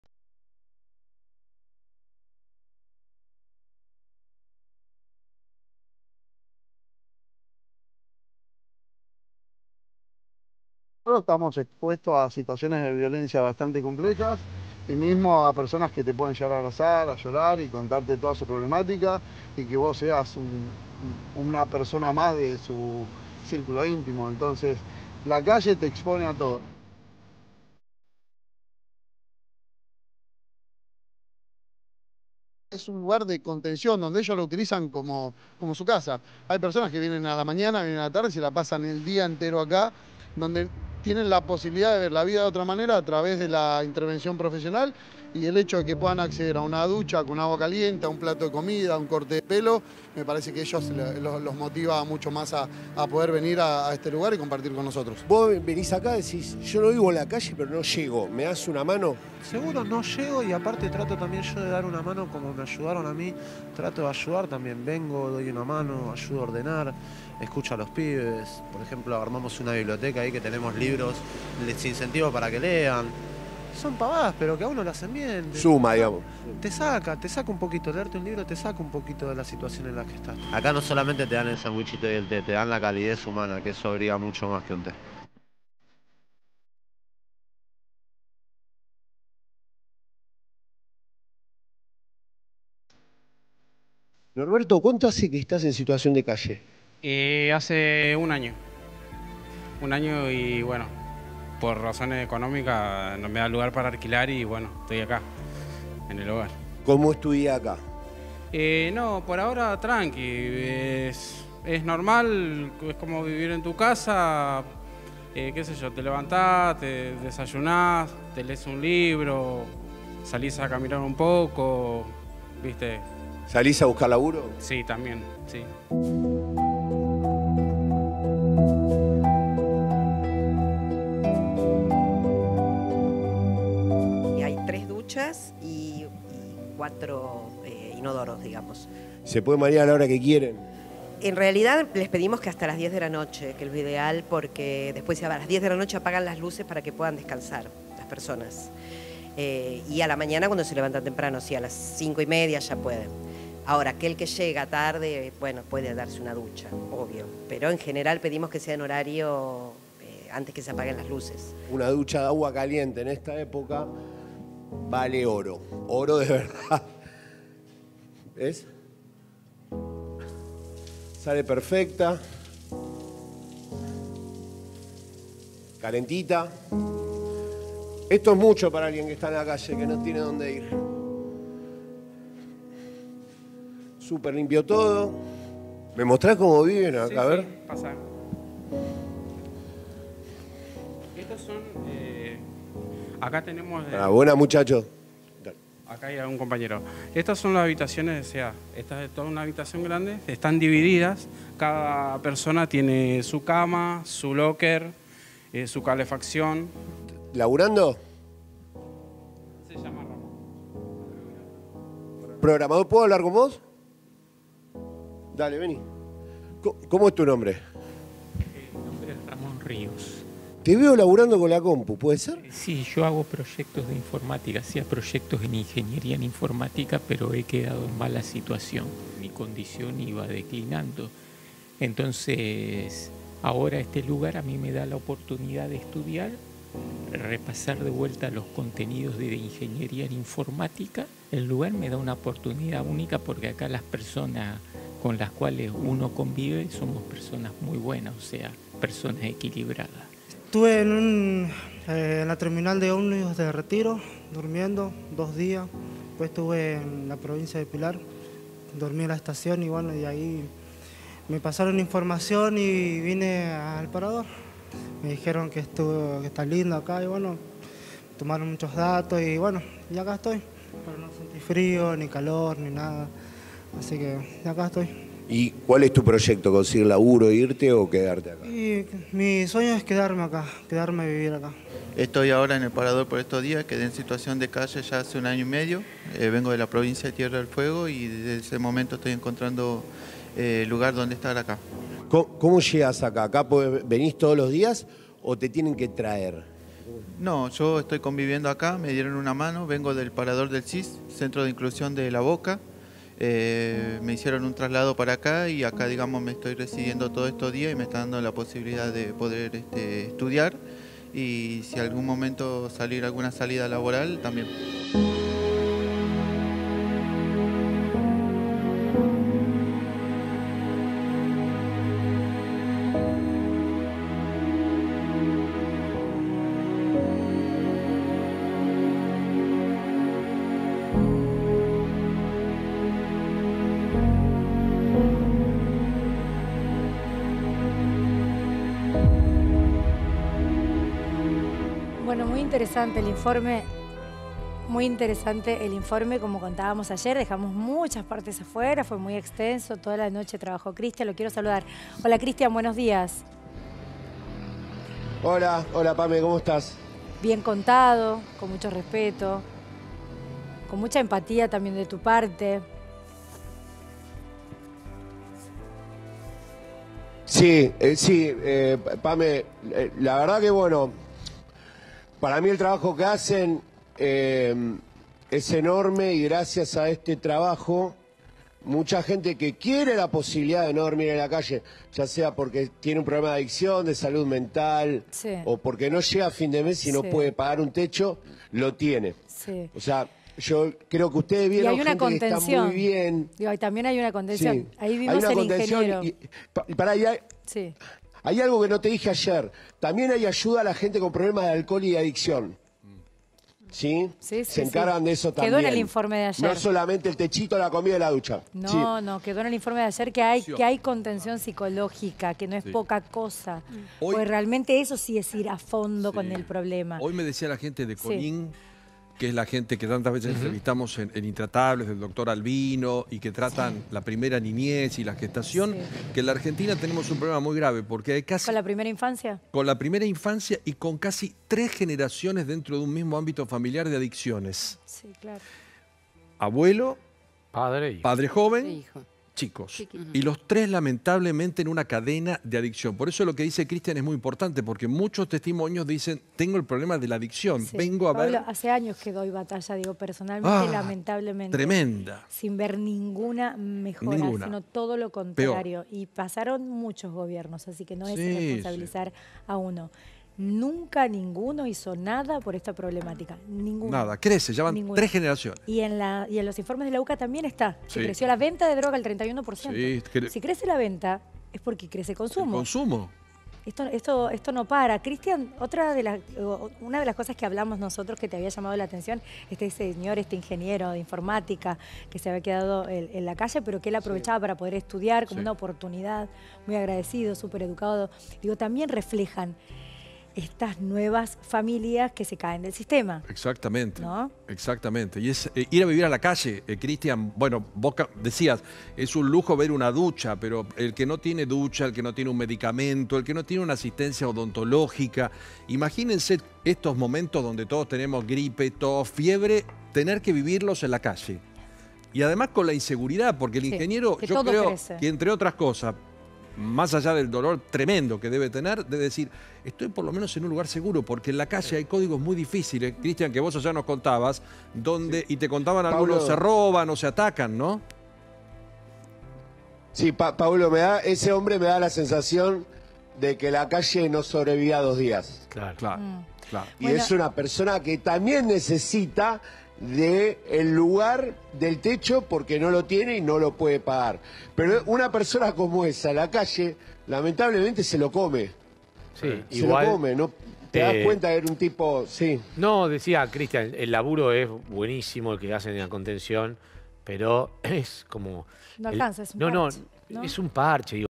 Sobrevivir al frío: cómo son los paradores por dentro
Así viven las personas en situación de calle que buscan refugio.